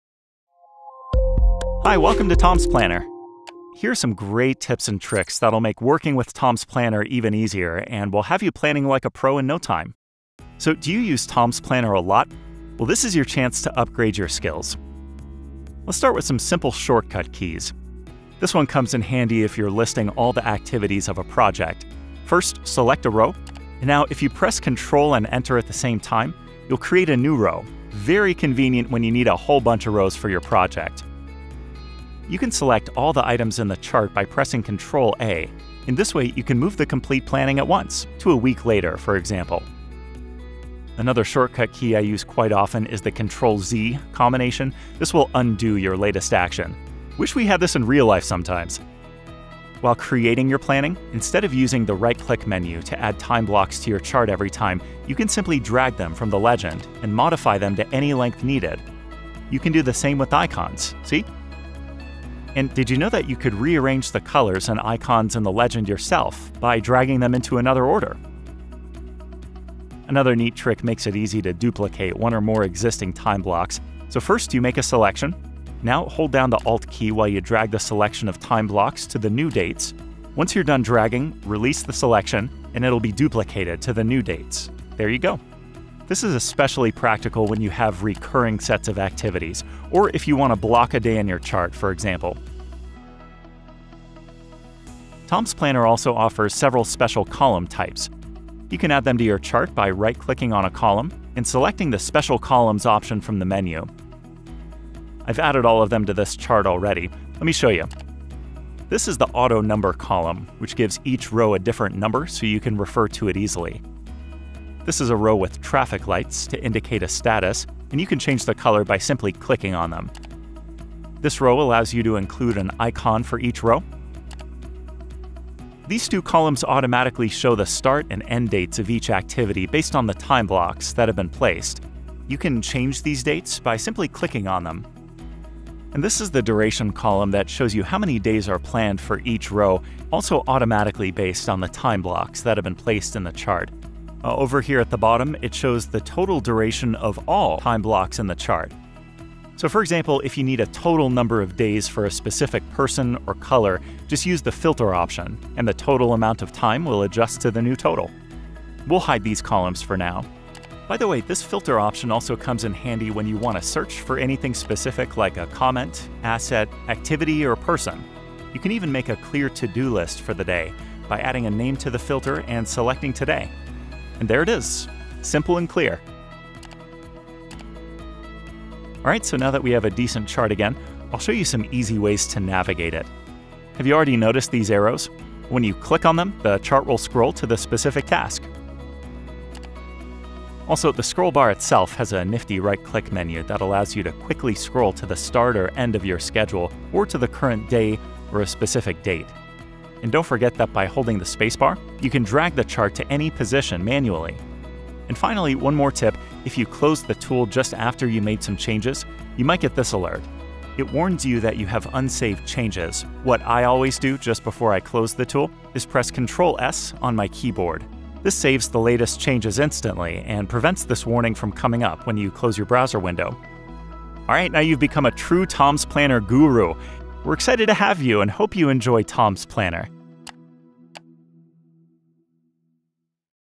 Tom's Planner Tips & Tricks
A must-watch for everyone that uses Tom's Planner on a regular basis. Tom's Planner is full of little functions and features that are not apparent at first glance but can really make your life easier.